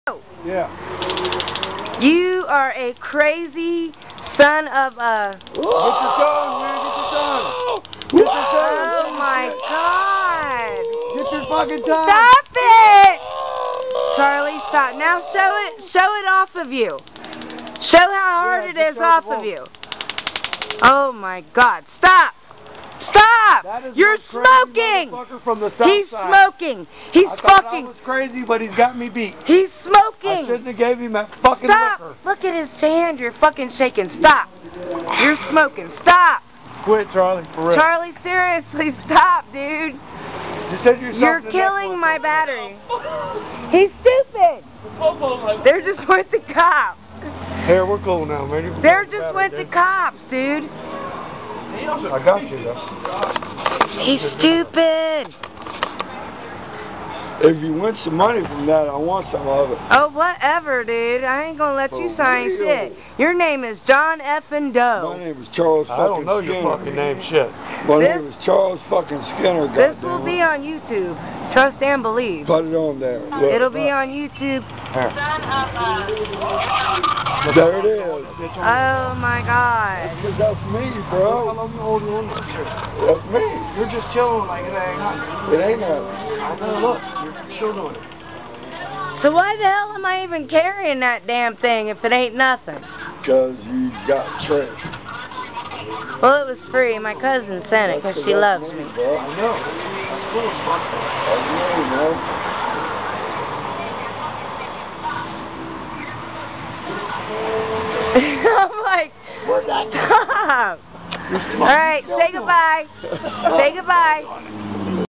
The Taser Man
My cousin sent me a taser to keep me safe on my bus trips. I'd better HOPE I don't run into a guy like THIS during my travels... lol!

This is my goofball friend, Charlie Skinner.. He no longer has hair on his arm on that spot...